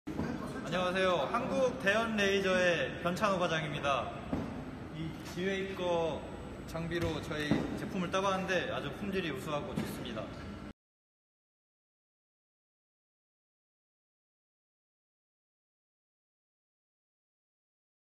Korea customer feedback for G WEIKE fiber laser metal cutting machine
See feedback from Korea customer of G.WEIKE fiber laser cutting machine, hope it can help you know more about G.WEIKE.